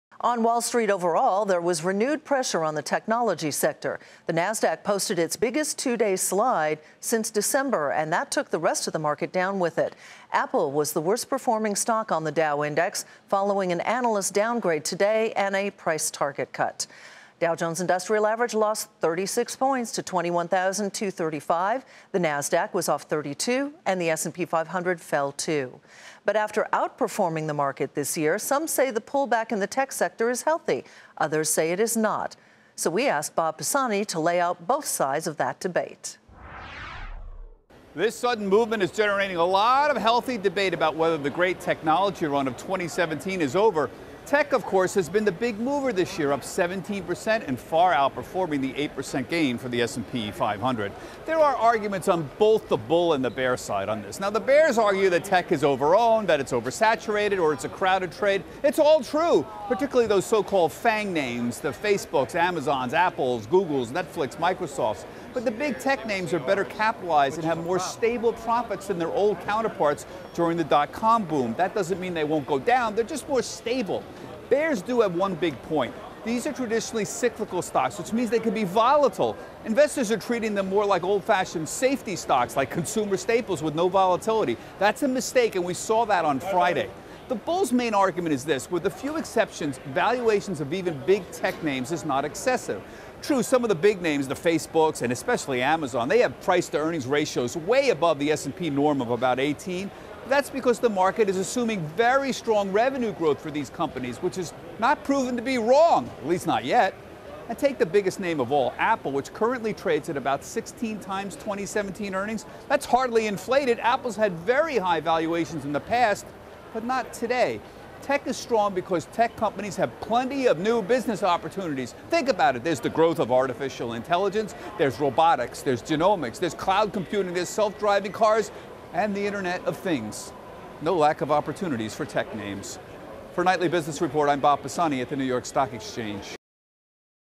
Tech bull v. bear debate
Some say tech's recent pullback is healthy, while others say it's not. Bob Pisani lays out both sides of that debate.